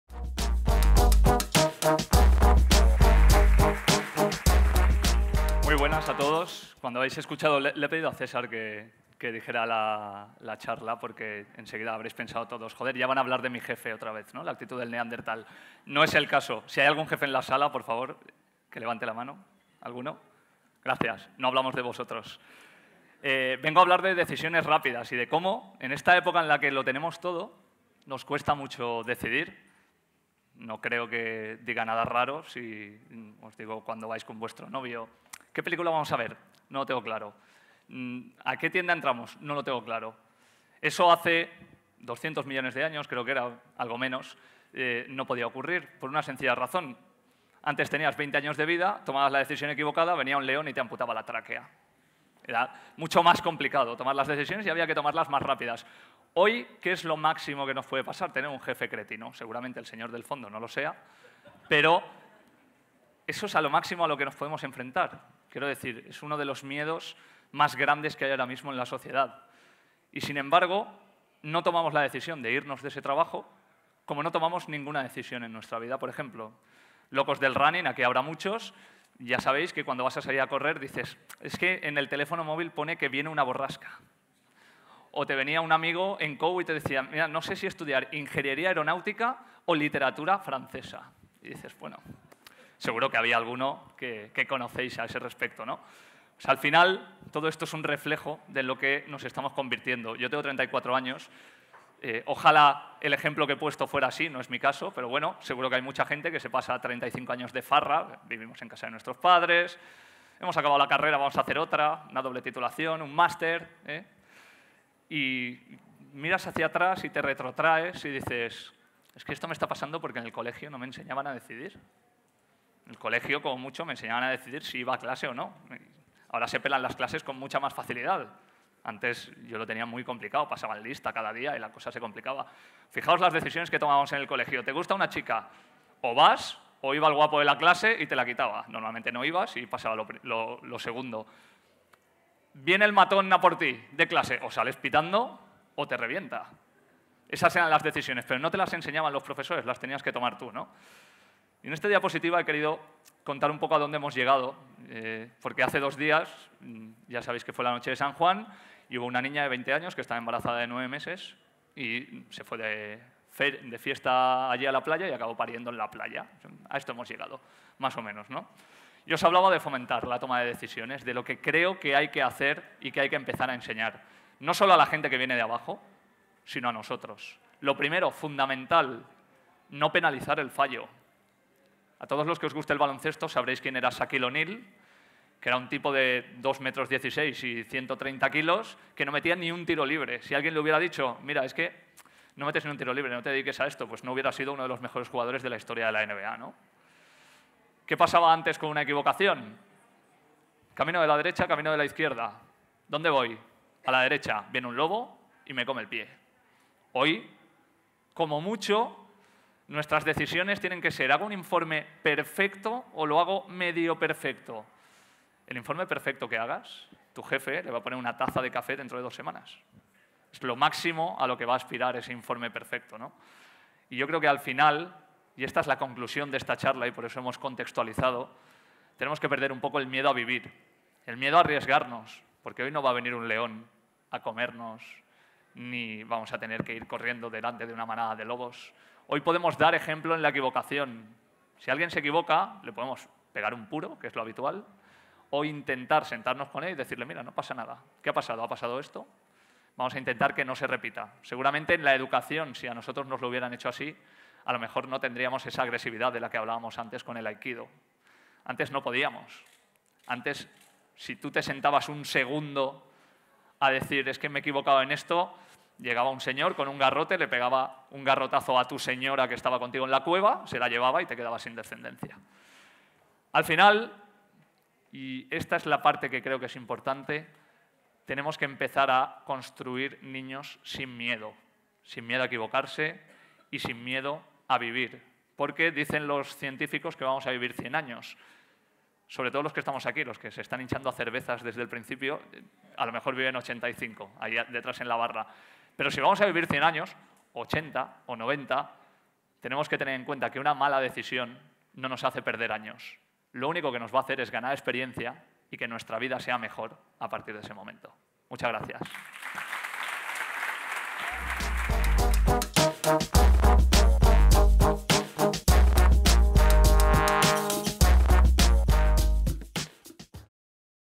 Ignite Valencia #4 - David Blay: La actitud del Neandertal
Ignite Valencia #4 se celebró el 27 de Junio de 2013.